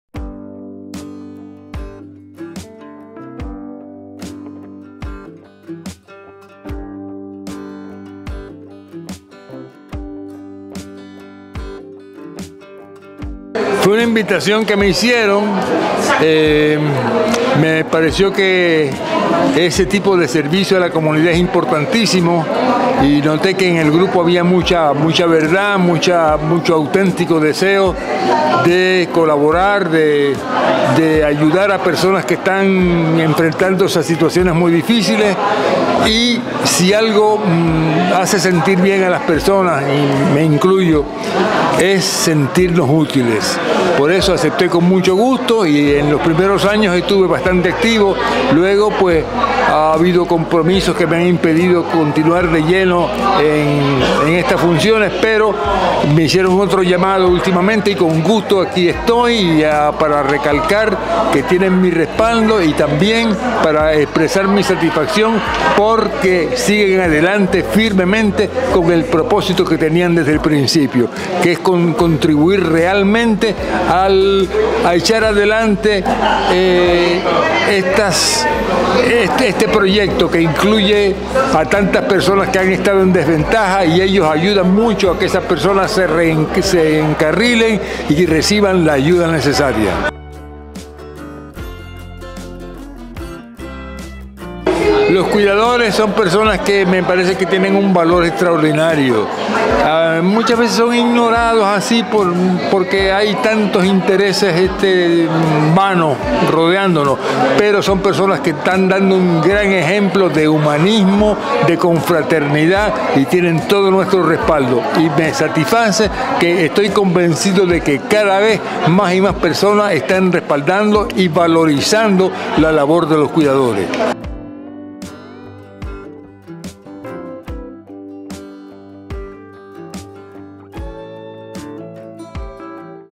Entrevista Jacobo Morales
Jacobo Morales, cineasta, actor, poeta, director y animador puertorriqueño, comparte la importancia de contribuir educación y sensibilización sobre el alzhéimer y otras condiciones de salud mental.